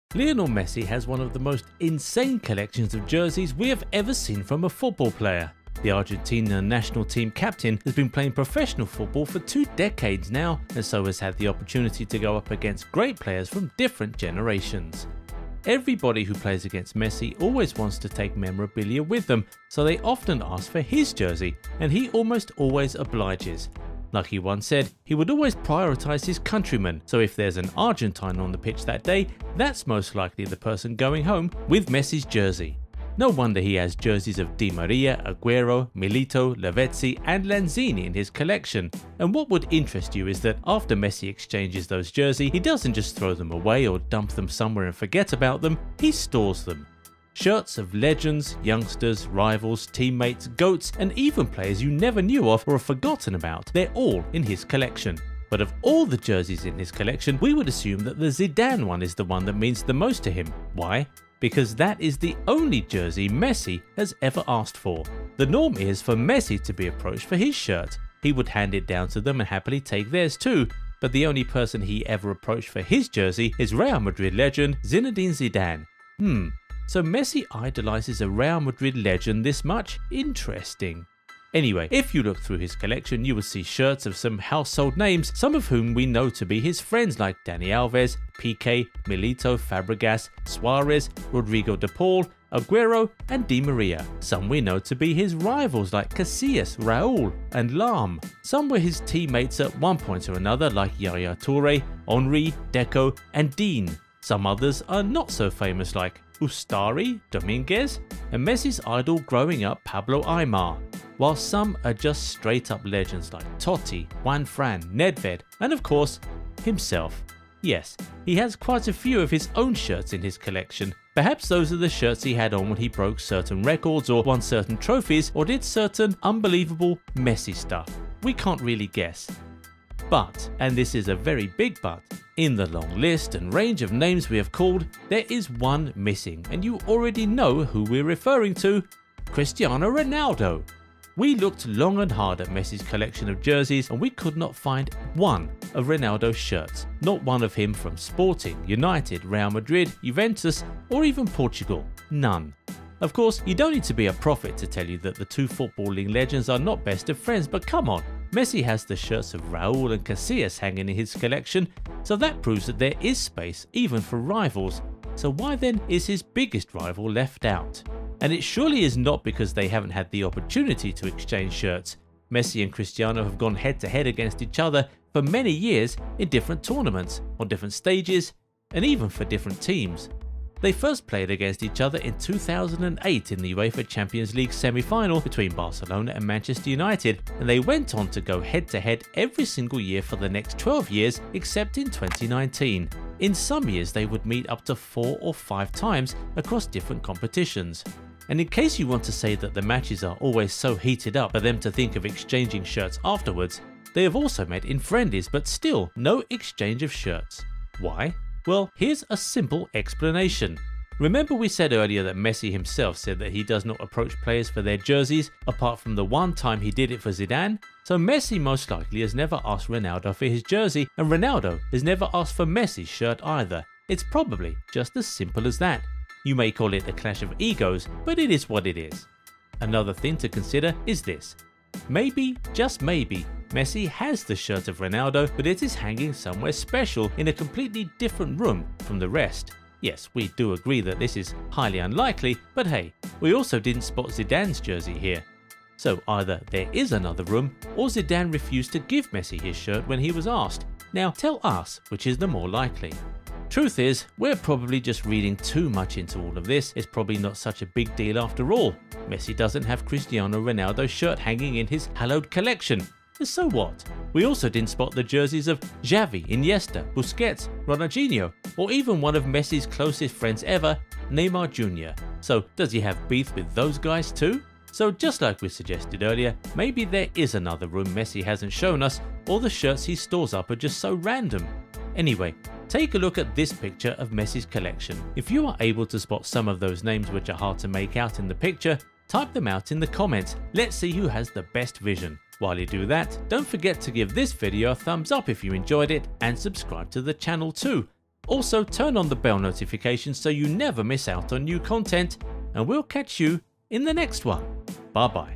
Why Does Messi Not Have Ronaldo's Jersey In His Jersey Collection?
Lionel Messi has one of the most insane collections of jerseys we have ever seen from a football player. The Argentina national team captain has been playing professional football for 2 decades now, and so has had the opportunity to go up against great players from different generations.

Everybody who plays against Messi always wants to take memorabilia with them, so they often ask for his jersey, and he almost always obliges. But like he once said, he would always prioritize his countrymen. So, if there’s an Argentine on the pitch that day, that’s most likely the person going home with Messi’s jersey.

No wonder he has jerseys of Di Maria, Aguero, Milito, Lavezzi, and Lanzini in his collection.

And what would interest you is that, after Messi exchanges those jerseys, he doesn’t just throw them away or dump them somewhere and forget about them, he stores them up. Jerseys of legends, youngsters, rivals, teammates, GOATs, and even players you never knew of or have forgotten about, are all in his collection.